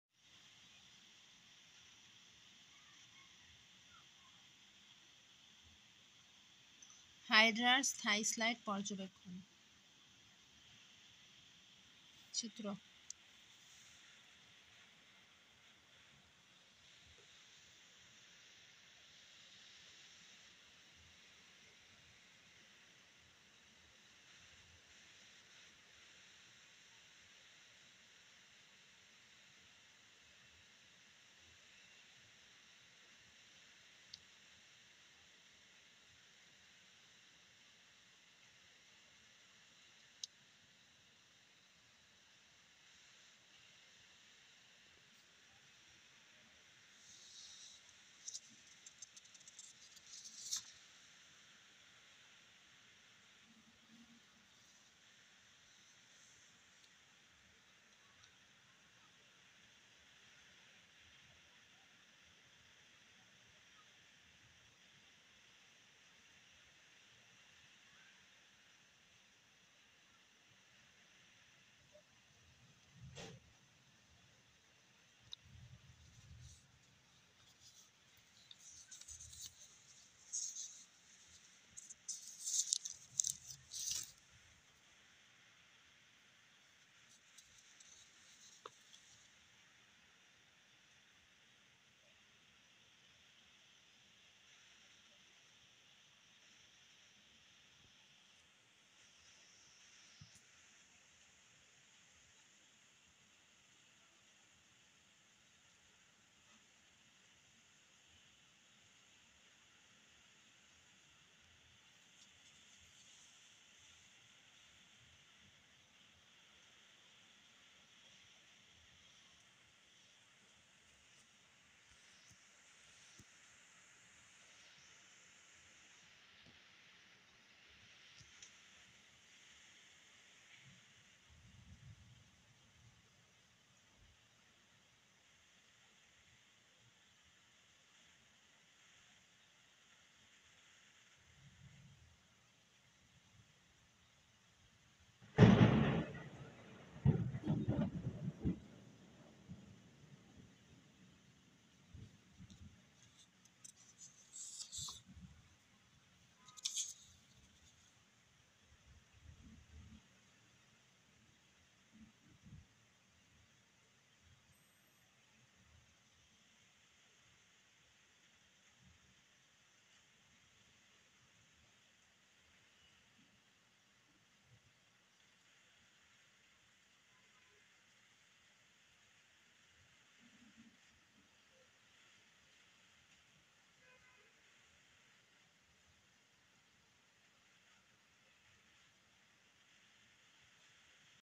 এইচএসসি জীববিজ্ঞান-২ ব্যাবহারিক২০২৩- হাইড্রার স্থায়ী স্লাইড পর্যবেক্ষণ
► for better experience, watch in 1080p
► please don't re post my hard work

► INDIVIDUAL PRACTICAL LINKS
►এইচএসসি জীববিজ্ঞান-১ পত্র ব্যাবহারিক ২০২৩

►এইচএসসি জীববিজ্ঞান-২ পত্র ব্যাবহারিক ২০২৩